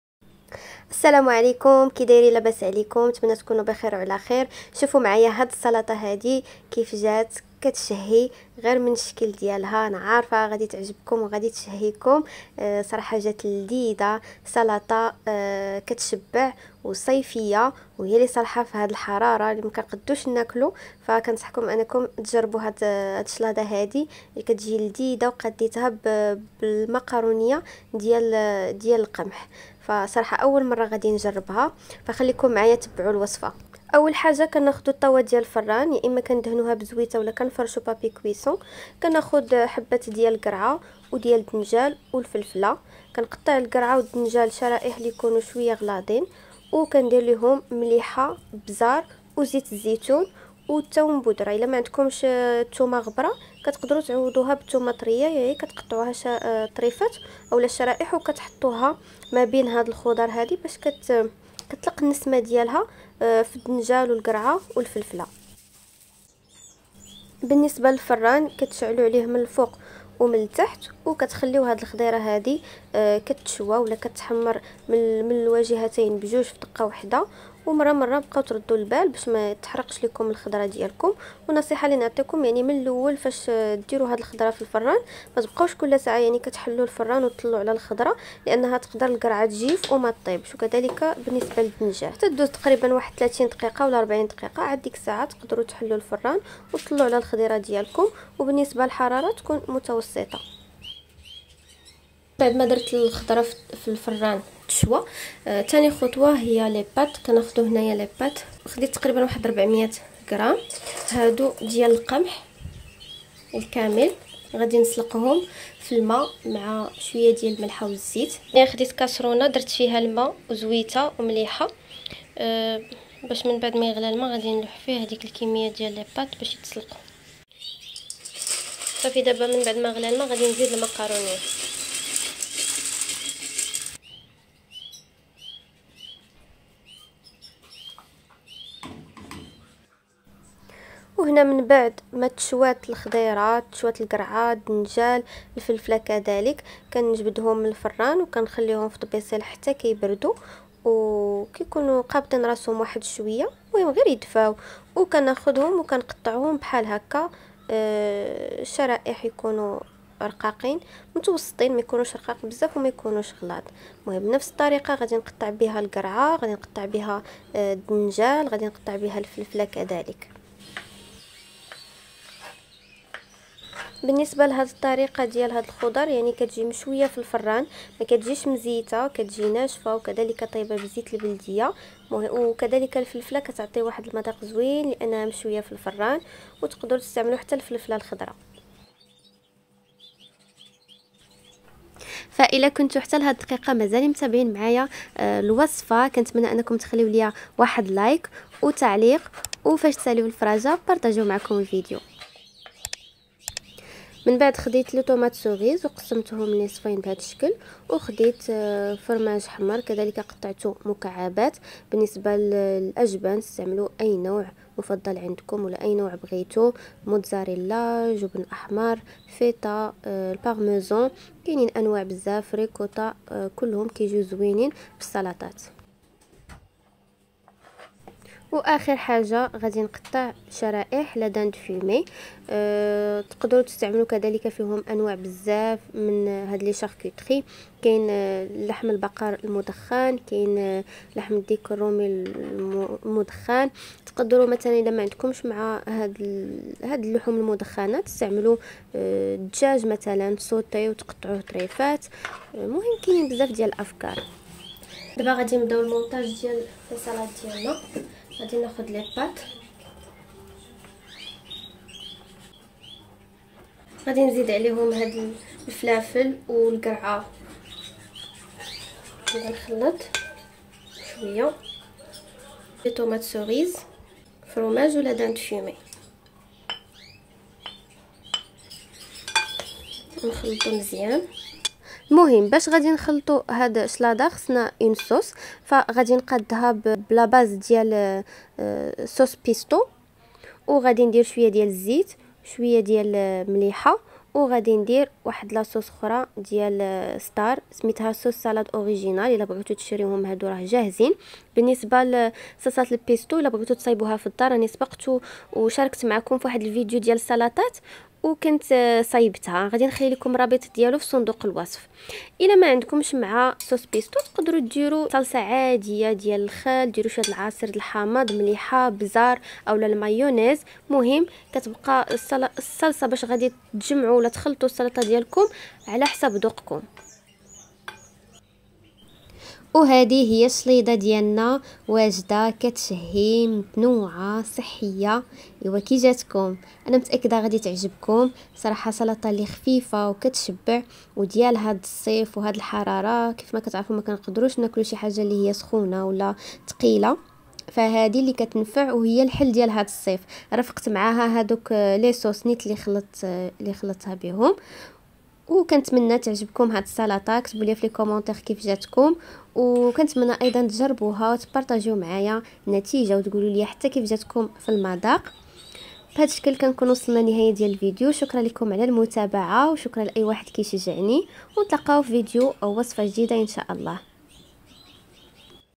ألذ سلطة🥗 تقدري تحضري هاد الصيف ساهلة صحية ولذييييييذة وكتشبع 😉💪
طريقة تحضير صوصو بيستو🌱 : غادي تحتاجو أوراق الريحان( basilic ) + أوراق المعدنوس+ م ك كركاع او لوز محمص + 2 فصوص ثوم+ ملح ،ابزار + زيت زيتون
نضع المكونات في الخلاط الكهربائي ،نطحن جيداا ونزيد زيت زيتون إدا كان الخليط شوية تقيل وبالصحة والراحة